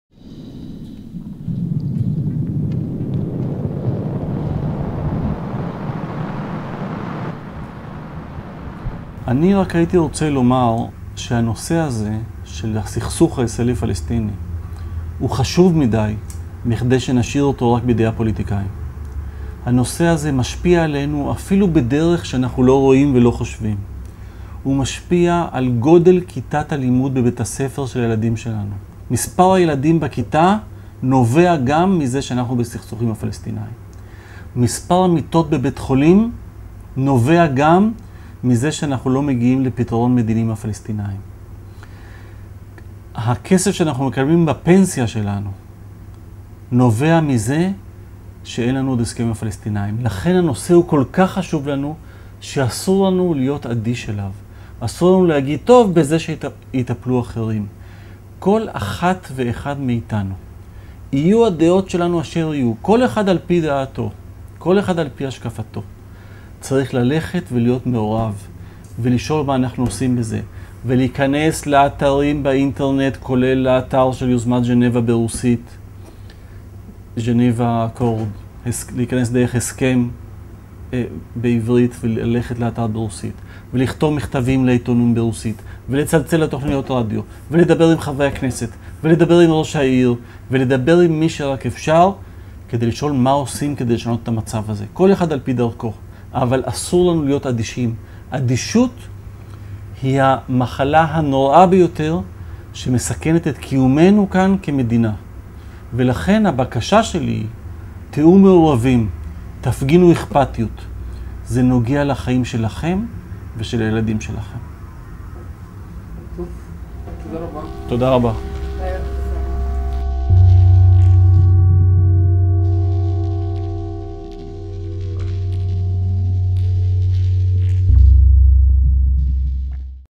גדי בלטיאנסקי: אסור להיות אדישים - Миротворчество
Обращение Гади Балтиански к зрителям: Не будьте безразличны к конфликту. Слишком большое влияние на нашу жизнь он оказывает, что бы оставлять его решение только политикам.

נושא האחרון בראיון
מראיין: קונסטנטין גרוסמן
מפיקה: רעיה המאיר
צילום ועריכה: heskem.tv

Завершая это интервью, я хочу сказать, что тема палестино-израильского конфликта слишком важна, что бы оставлять её только политикам.

Этот конфликт влияет на нашу жизнь даже там, где мы меньше всего ожидаем этого: он влияет на количество учеников в классе, на количество больничных коек, на наши пенсионные сбережения  всё это и многое другое связанно с конфликтом. Эта тема настолько важна, что просто запрещено быть безразличными к ней.  Нельзя отмахнуться: «Пусть политики этим занимаются». Каждый, в соответствии со своим мировоззрением, должен прилагать все усилия для того, что бы помочь разрешить этот конфликт.  Нужно быть вовлечённым, изучать этот вопрос со всех возможных точек зрения, искать информацию на профессиональных информационных ресурсах, писать письма в газеты, звонить на радио, говорить с депутатами Кнессета и представителями общественности  делать всё для того, что бы изменить эту ситуацию. Но нельзя быть безразличными  безразличность опасна для гражданского общества. Моя просьба  будьте вовлечёнными в решение конфликта, его продолжение влияет на вашу жизнь и на жизнь ваших детей.

Спасибо.